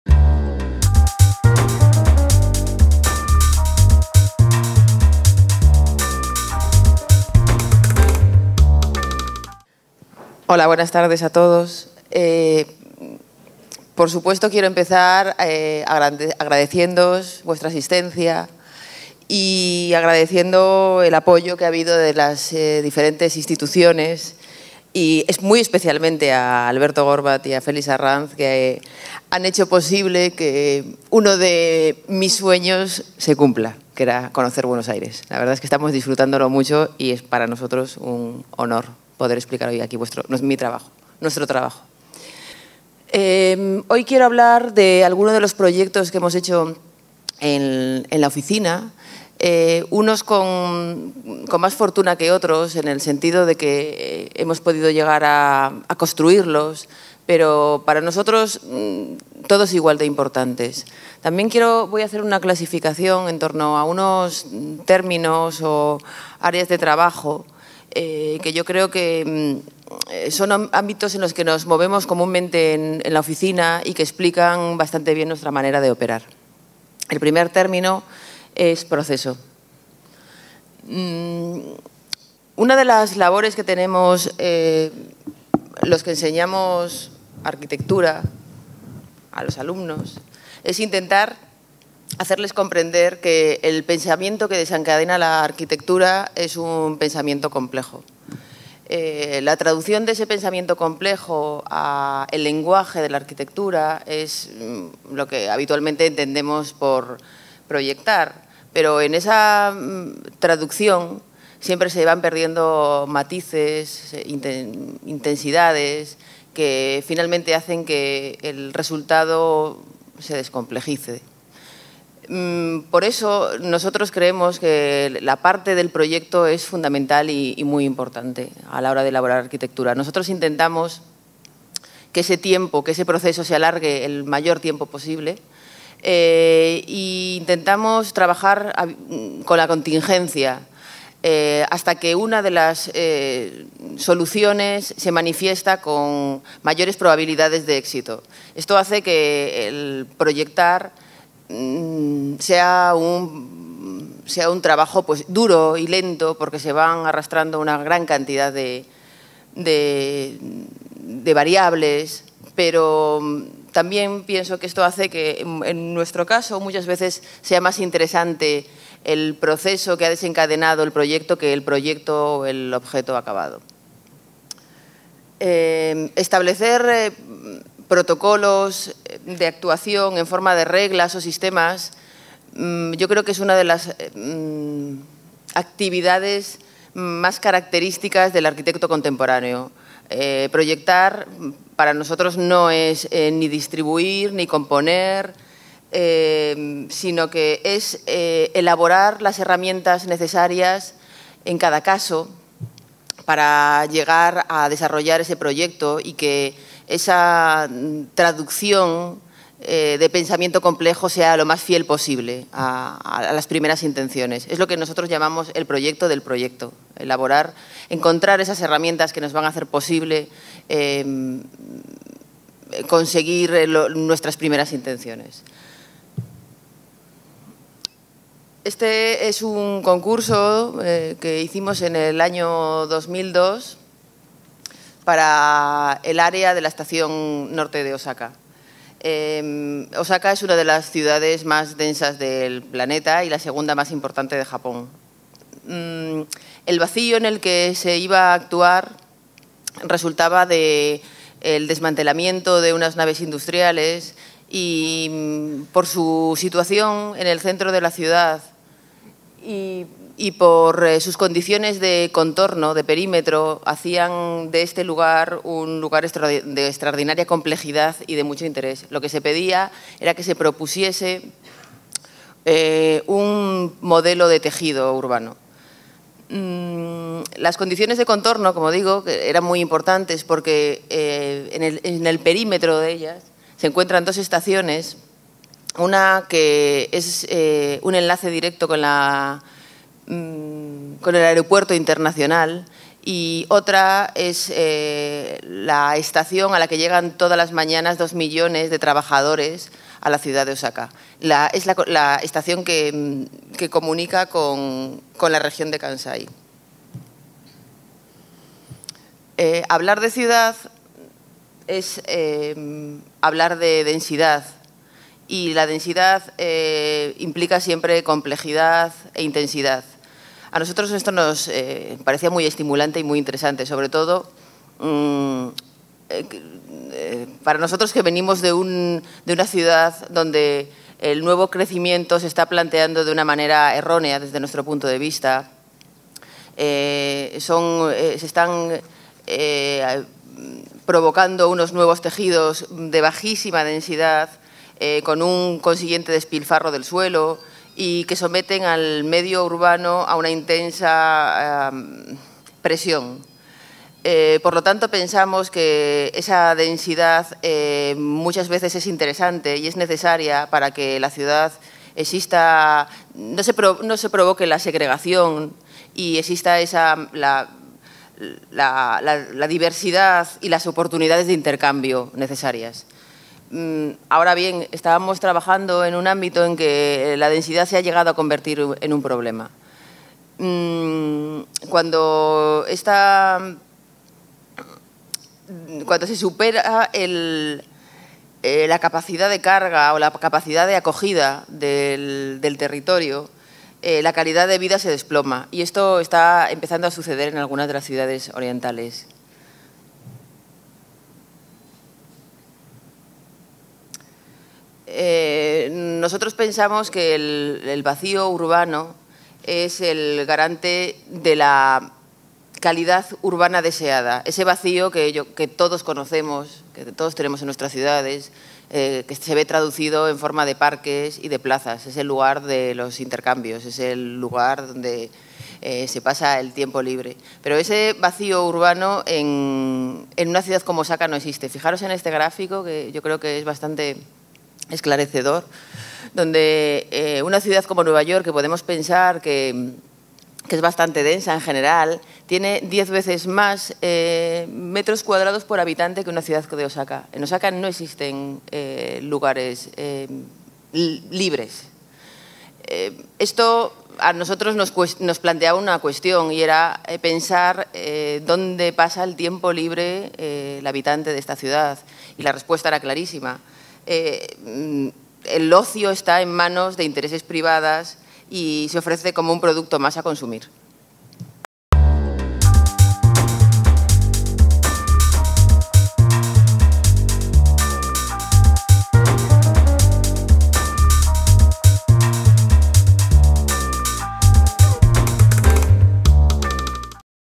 Arq. Victoria Acebo - 01/13
Encuentros de Arquitectura AE
Conferencia Magistral de la Arq. Victoria Acebo
Parte 01 de 13.
Mayo de 2005, Buenos Aires, Argentina.